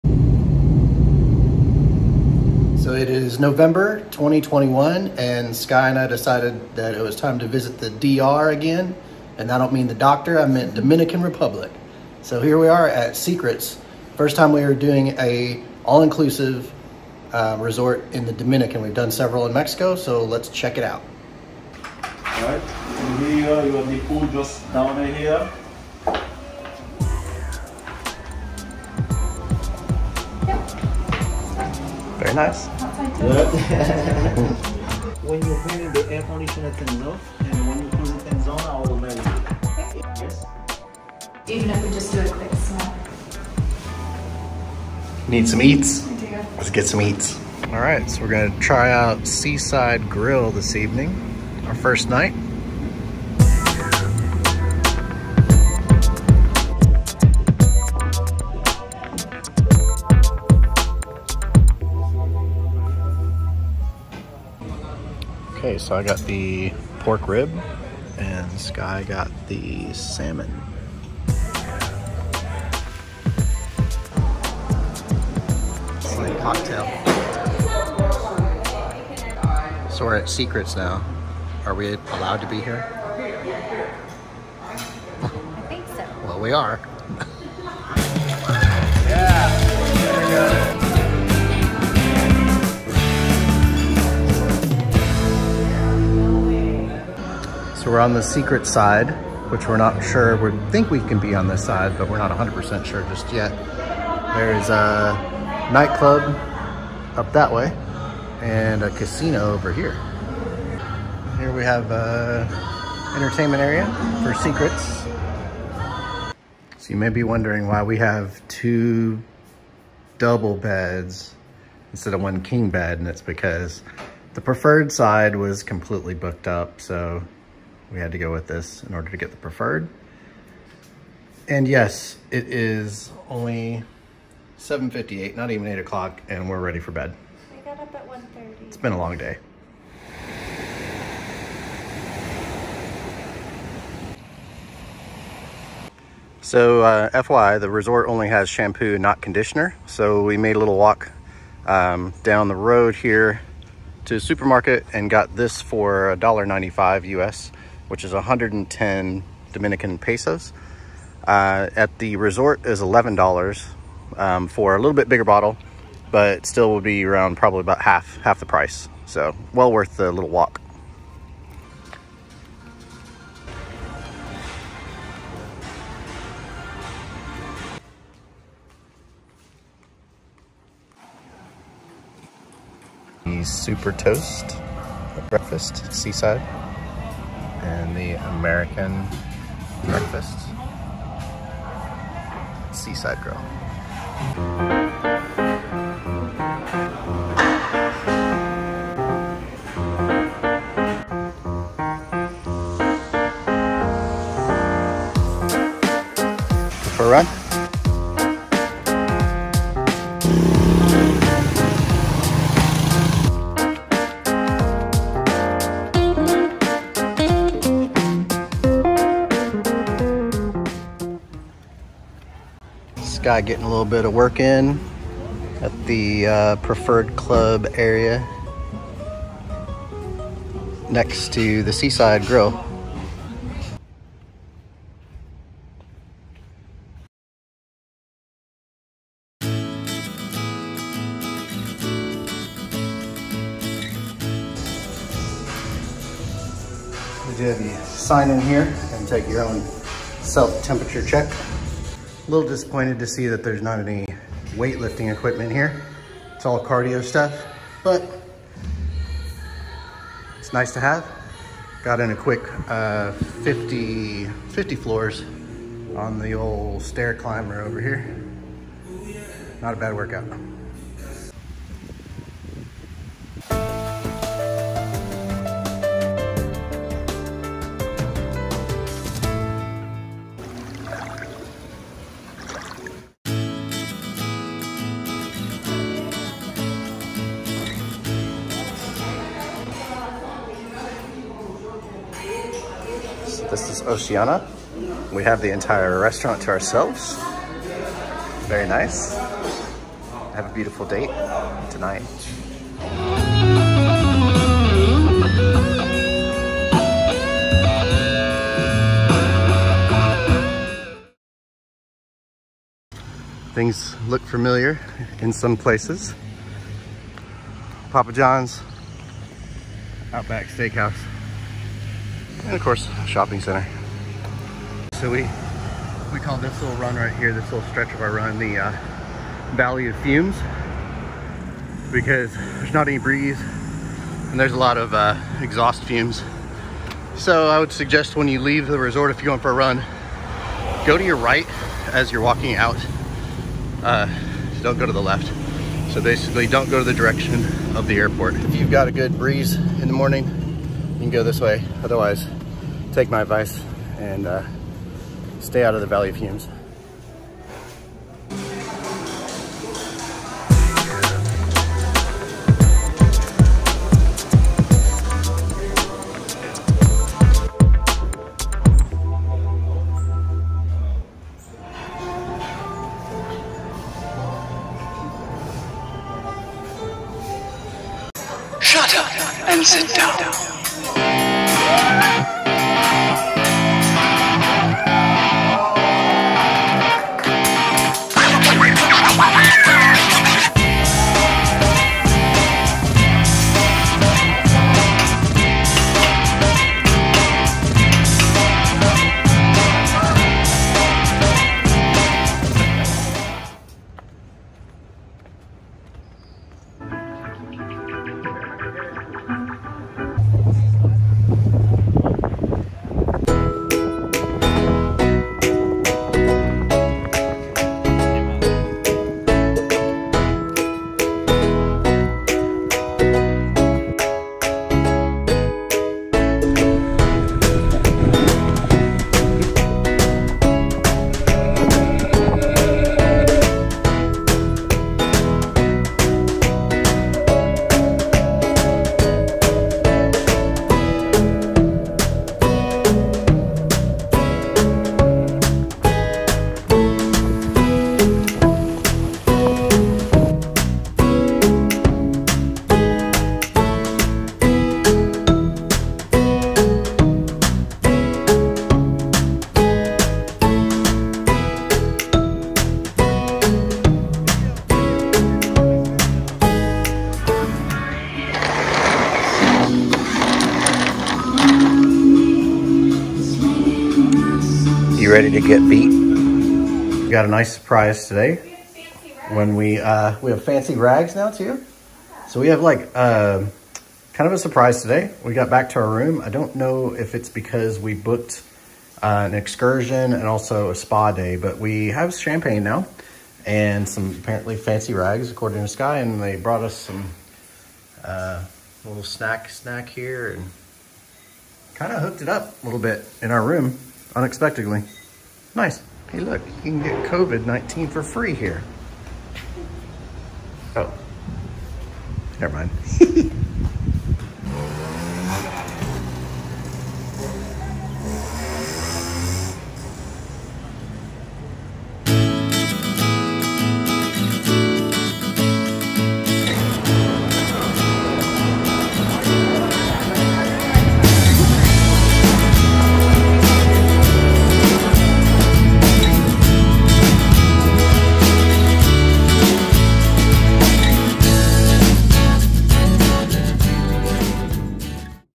Dreams Royal Beach, Punta Cana, Dominican Republic travel tips - November 2021
Travel along with us as we visit our first all inclusive resort outside of Mexico.  Dreams Royal Beach near Punta Cana, Dominican Republic is located next door to Secrets (an adults only all-inclusive).

We also booked a tour of the Marinarium.  This excursion provides an opportunity to swim with sharks and stingrays, kayak, paddle board, or just relax and enjoy a relaxing massage with a nice ocean breeze above a coral reef offshore.